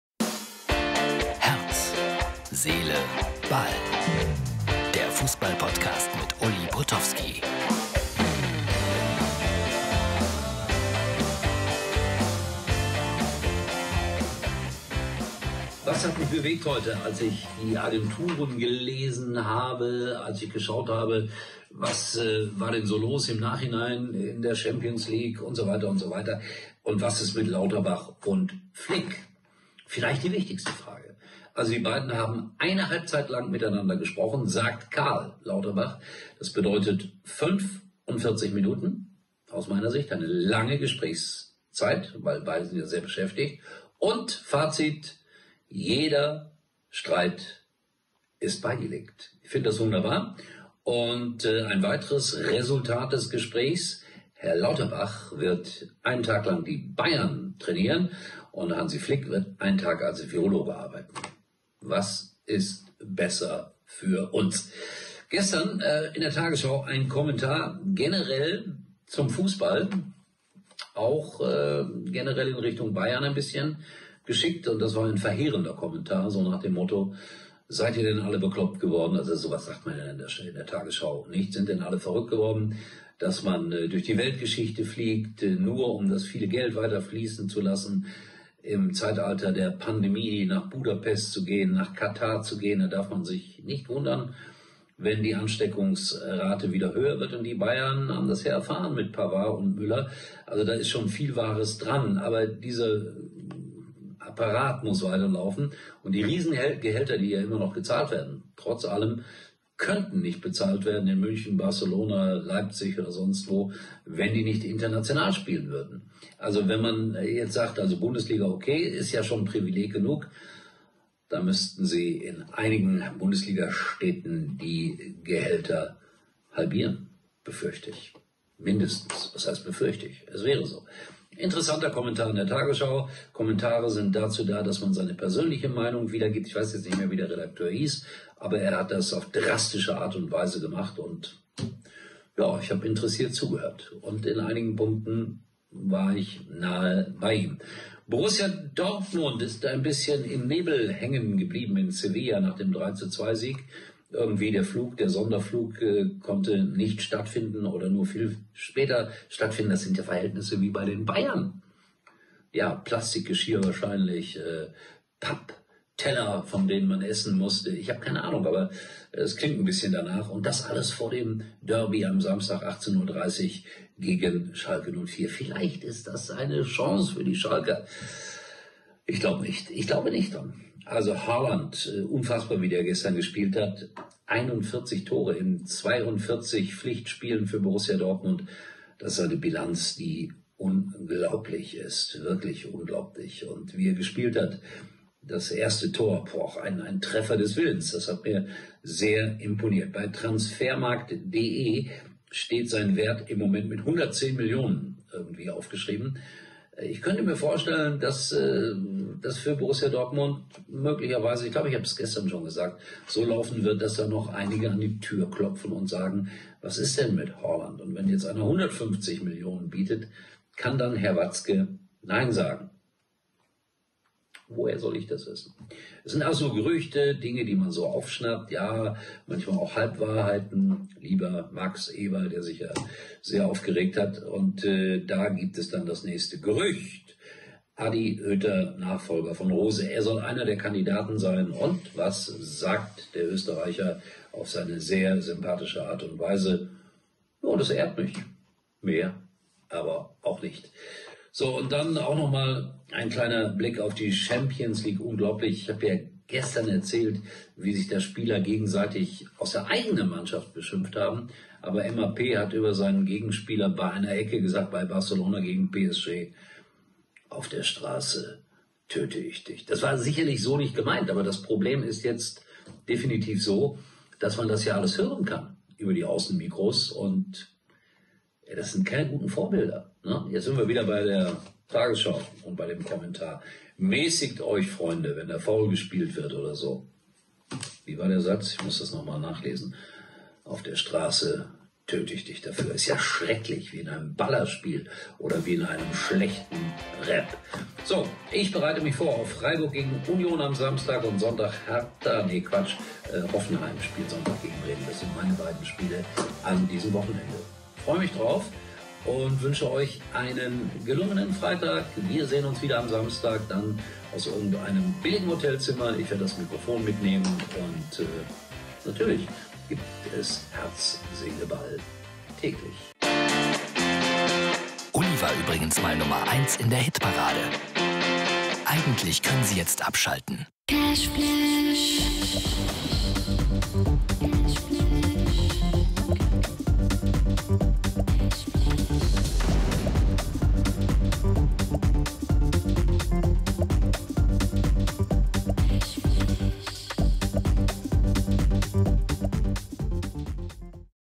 Herz • Seele • Ball - Folge 551 - Herz Seele Ball - Ulli Potofskis täglicher Fußballpodcast
Der BVB stochert im Nebel und
„unfassbare Worte“ auf dem Fußballplatz...